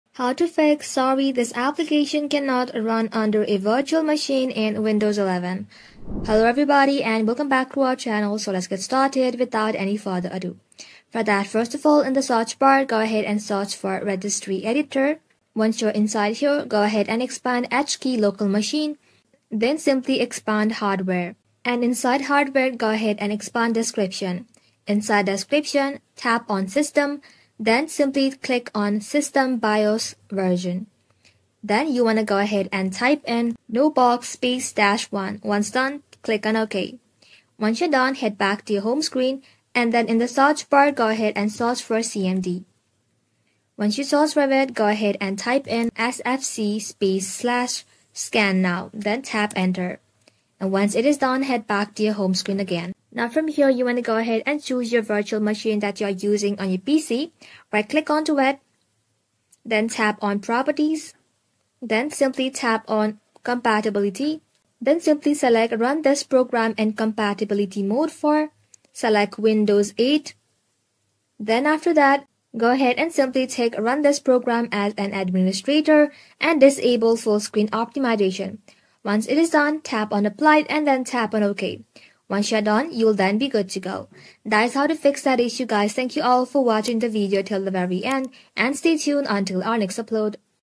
How to Bypass "Sorry this application cannot run under a Virtual Machine" error
How To Fix "Sorry, This Application Cannot Run Under a Virtual Machine" on Windows 10/8/7

Unlock the secrets of fixing the "Sorry, this application cannot run under a virtual machine" error on Windows 10/8/7 with this comprehensive guide, designed to help you bypass virtualization restrictions effortlessly. Whether you're running software on a virtual machine or encountering a false detection, following these steps ensures a smooth fix.

In this video:
0:00 - Introduction
0:05 - How To Fix "Sorry, This Application Cannot Run Under a Virtual Machine" on Windows 10/8/7

We’ll walk you through the specific actions needed to resolve this issue. Learn how to disable virtualization detection, modify system settings, and use Registry Editor or Group Policy to bypass VM restrictions. We'll also cover troubleshooting tips and alternative methods to ensure the application runs without errors.

Master the process of fixing the "Sorry, this application cannot run under a virtual machine" error effortlessly with this expert guide for 2025!